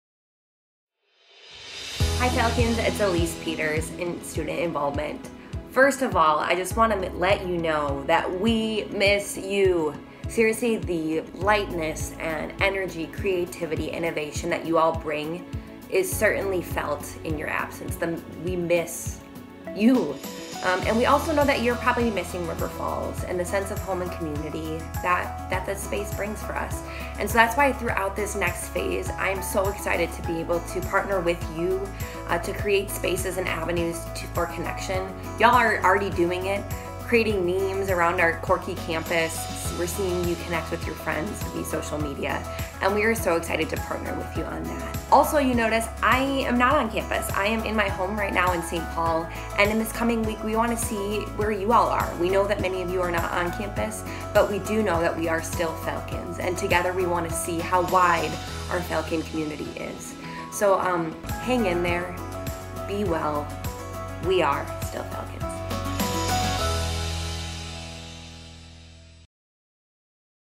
UW-River Falls Student Involvement - 'We Are Still Falcons!' - 20200330
Elise Peters, UW-River Falls Student Involvement Events and Activities coordinator, explains how UWRF wants to stay in touch with students as the semester resumes with many students away from campus and faculty using alternate methods of instruction. No matter where we are, though, what connects our UWRF family is very simple: We are still Falcons.